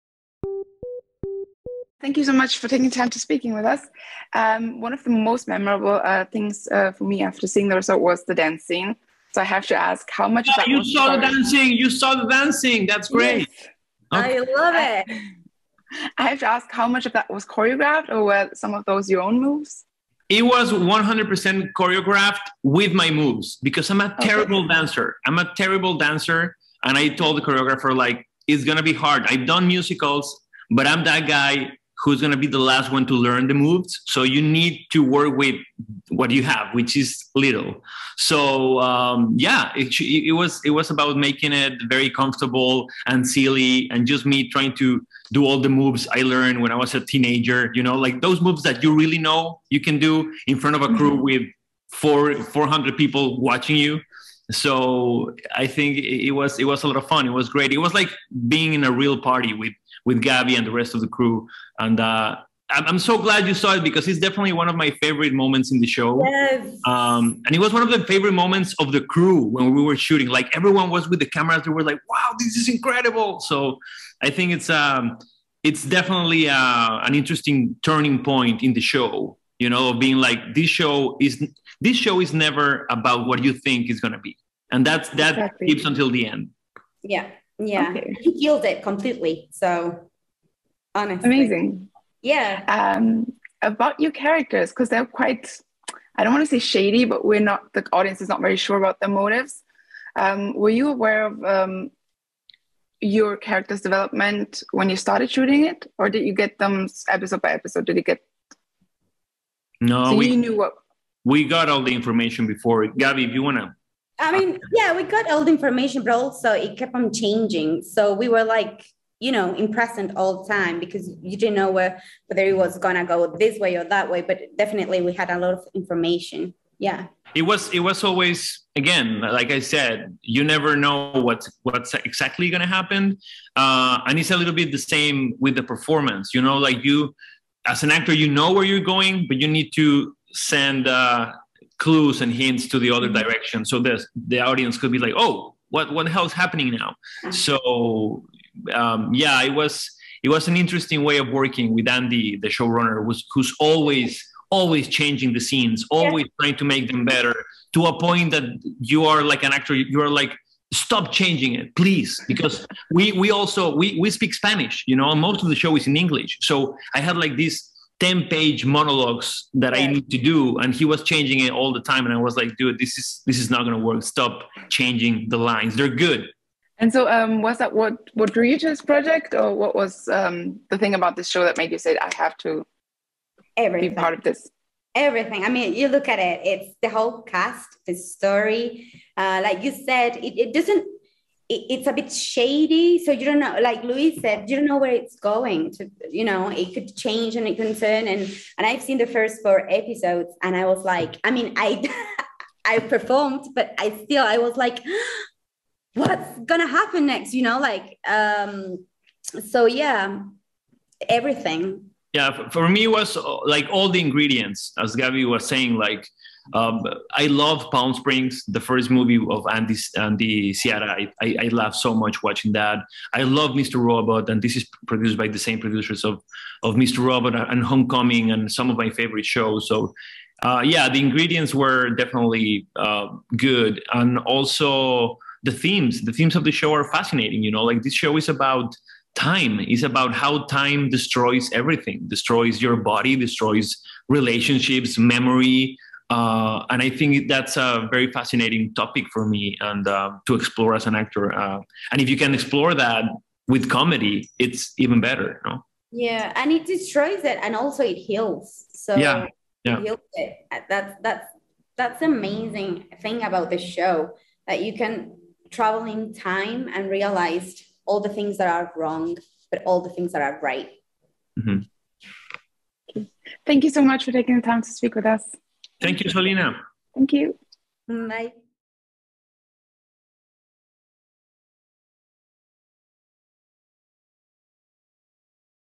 Gabriela Cartol & Luis Gerardo Méndez interview on The Resort
Interviewer: Selina Sondermann